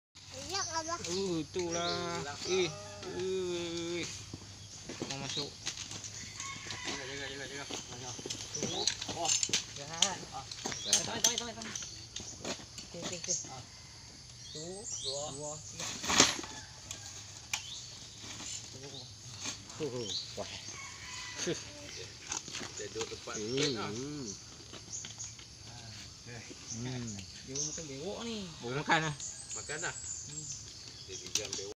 ular tedung selar gergasi
4x llp apm padang terap barjaya menangkap tedung gergasi